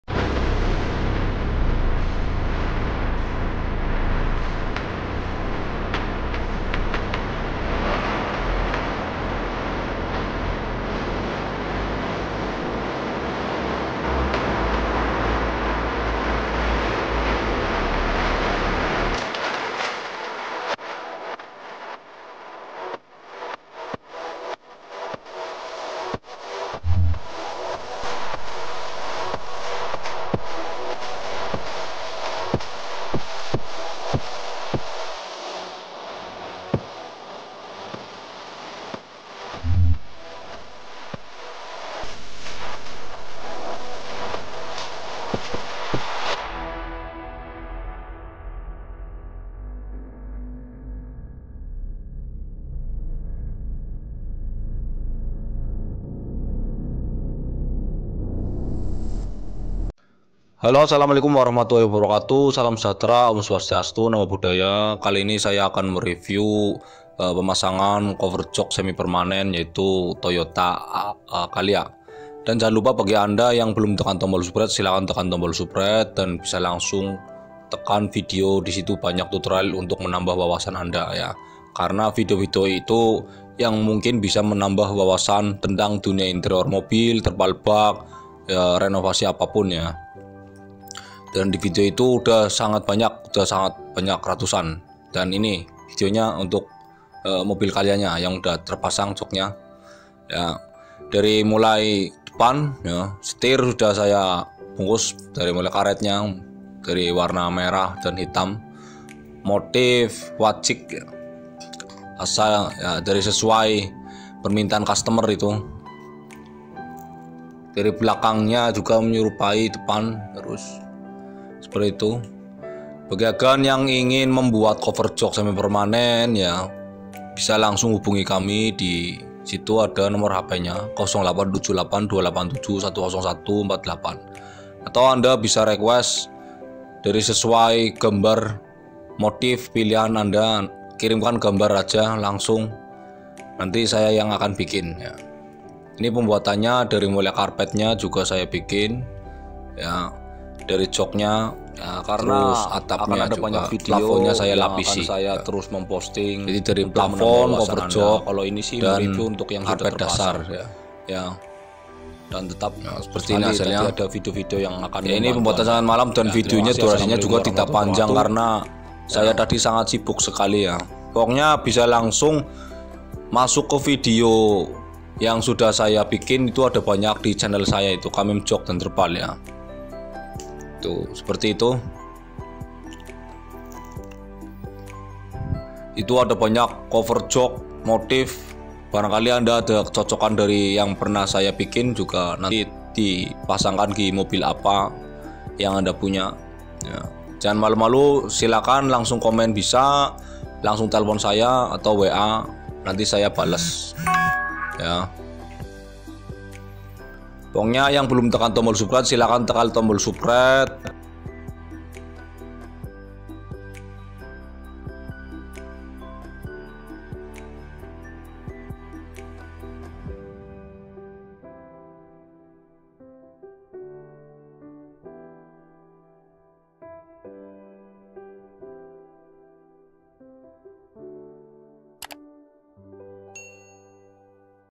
TEMPATNYA BIKIN COVER JOK MOBIL PLAFON MOBIL KARPET DASAR MOBIL DI TEGAL JAWA TENGAH
hallo assalamualaikum kali ini saya pos vidio pasang cover jok plafon karpet dasar untuk mobil calya/sigra jika kalian berminat  untuk bikin silakan langsung saja hubungi kami di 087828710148 hp/wa alamat kami di tegal jawa tengah